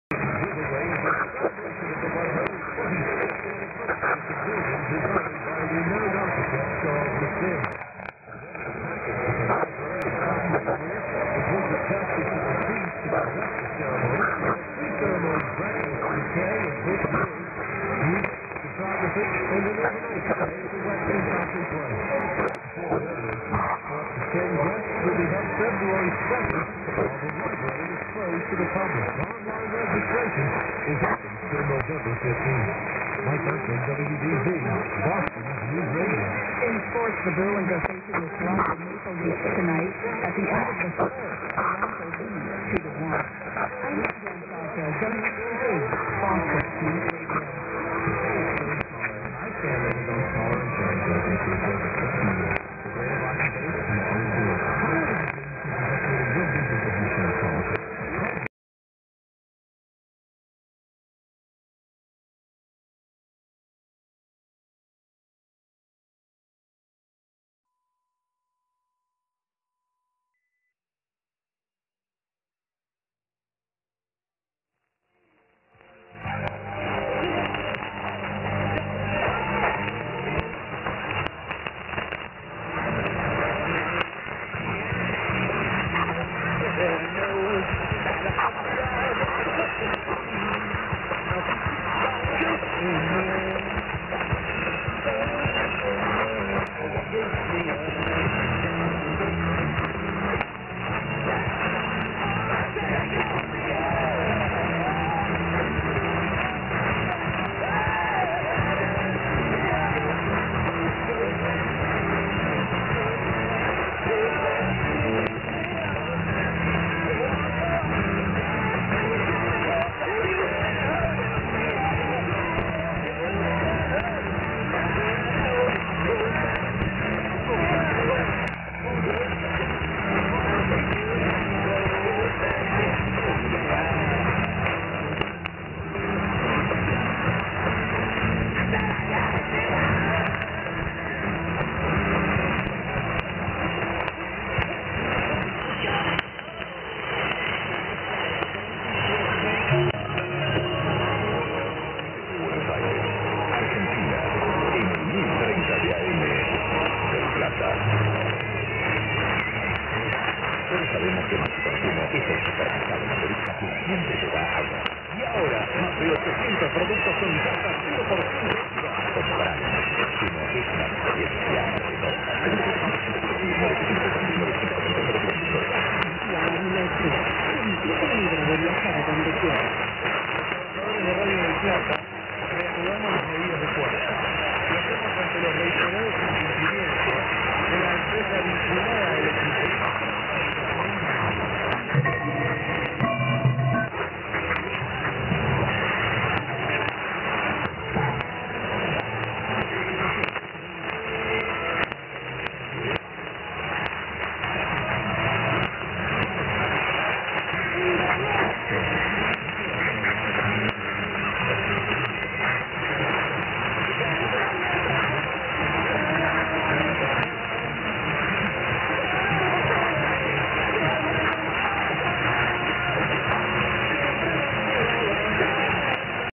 1030kHz , WBZ Newsradio 1030 (USA)+Radio Del Plata (ARG)
ELAD FDM-S2 (SDR Receiver),  antenna - Beverage 300m/305deg.
Western Siberia /Russia (QTH-NO15VM53) ID(WBZ) - 00.35.
ID-(Radio Del Plata) - 02.32.